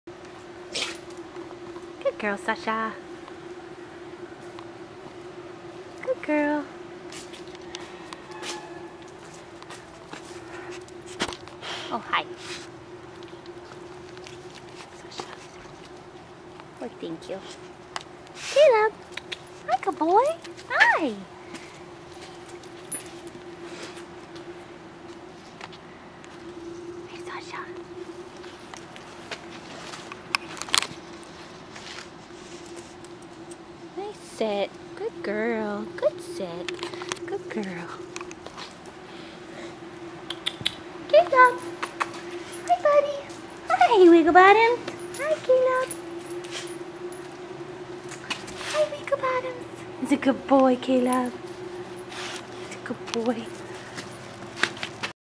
Meet Sasha a Terrier American Pit Bull currently available for adoption at Petango.com! 1/16/2014 7
EDAP- Sasha is a snugly and affectionate girl that has lots of energy!  She loves to play with other dogs that can keep up with her.  She is treat motivated and would benefit from confidence boosting and positive training.  Sasha would make a great running partner too, and is looking for someone with an active lifestyle.  She would do best without children under 9 years. Come meet this brindle beauty at PAWS today!